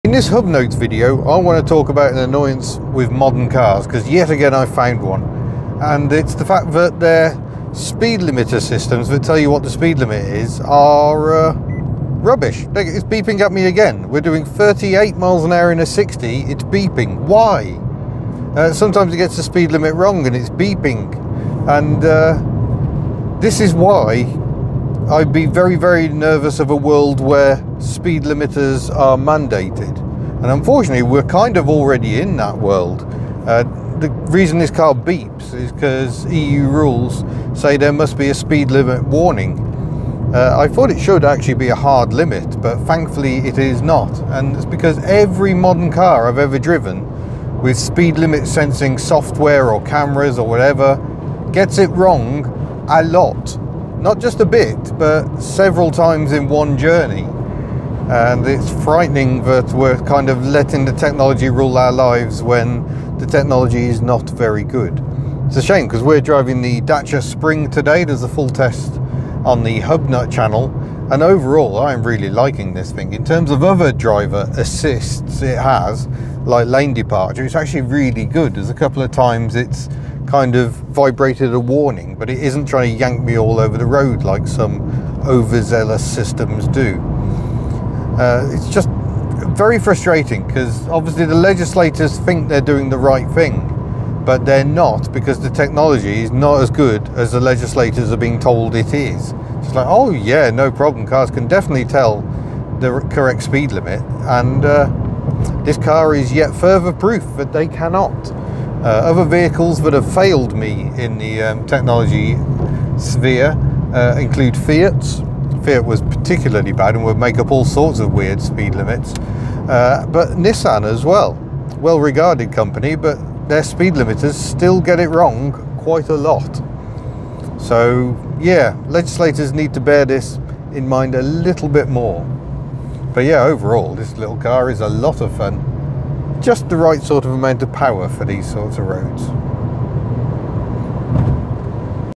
Why are car speed warnings so rubbish?
The Dacia Spring is not alone at being RUBBISH at identifying speed limits. Legislation mandates that speed limits are flagged by the cars, but the technology is terrible and very frequently gets it wrong. Legislators take note.

Also, I think the Dacia Spring has a programming error, so it was flagging up 37mph as a warning in a 60mph (37mph is 60kph). Technology we should not be relying on.